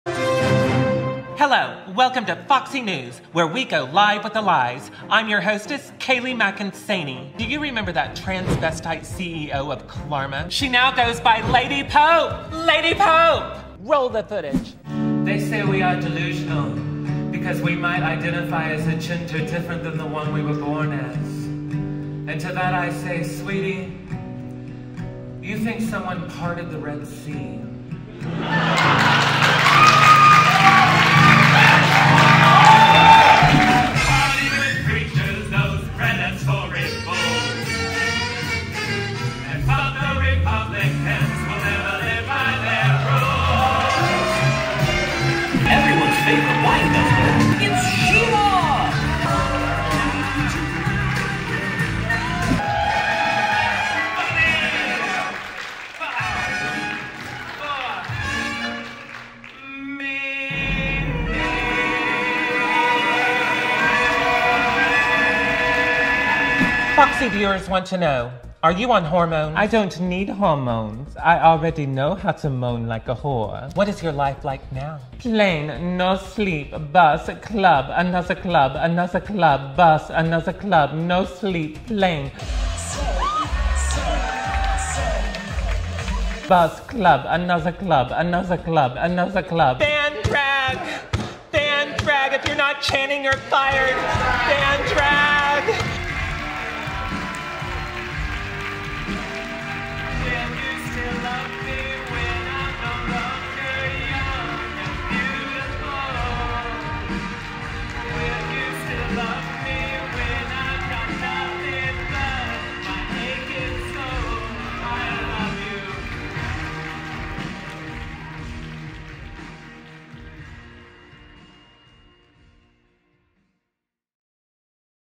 An Evening Without Kelly Mantle in LA
You asked for it, and now it's here! Tickets and VIP upgrades to see @thekellymantle's AnEveningWithoutKellyMantle in LA are now on sale! Don't miss your chance to see this talented Queen take the stage and steal your heart! kellymantlelive.com